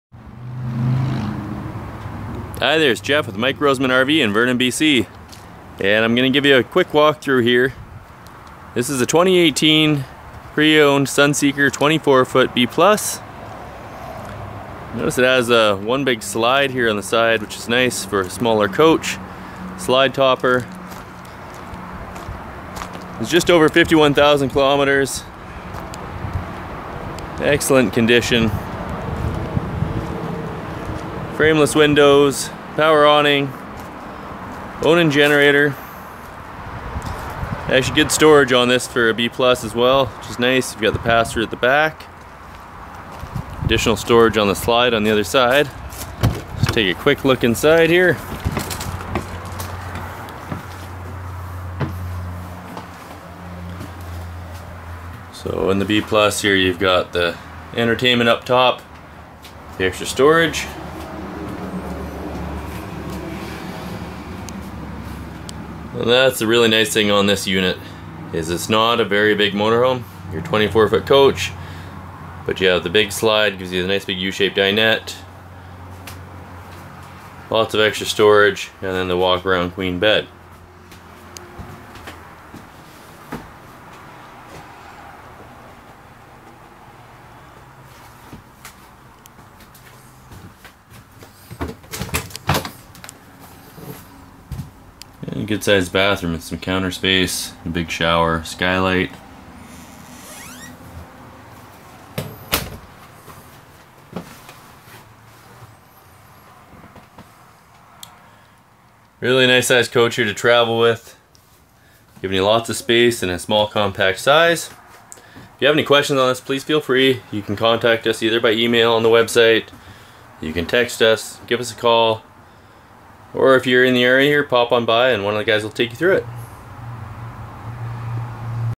Forest River Sunseeker 2018 Coach / 2017 Chassis
Here is our new 2018 Forest River Sunseeker Motorhome walk through video. Check out the amazing deals on the latest used RV's at Mike Rosman RV Sales, in Vernon BC.